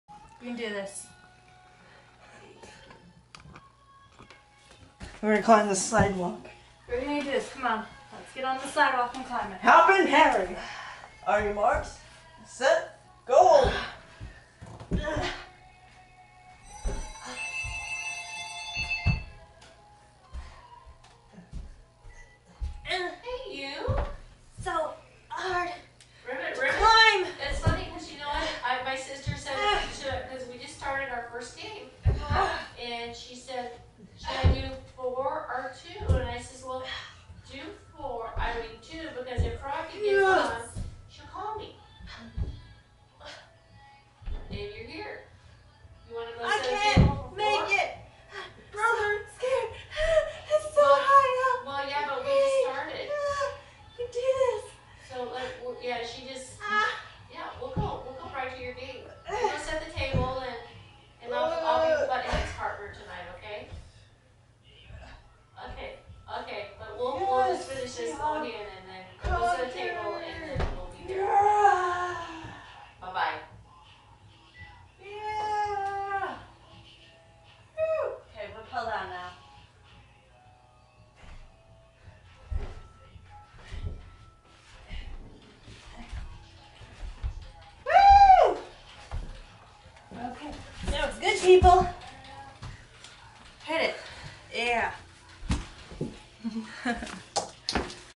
Climbing a SideWalk
62.Climb a sidewalk.
My friend say we didn't have sidewalks, so we climbed inside instead.

where we just blantly do the list. We only got halfway done through the day, however it was friken fun. If your really bored, I reccomend trying out sometime.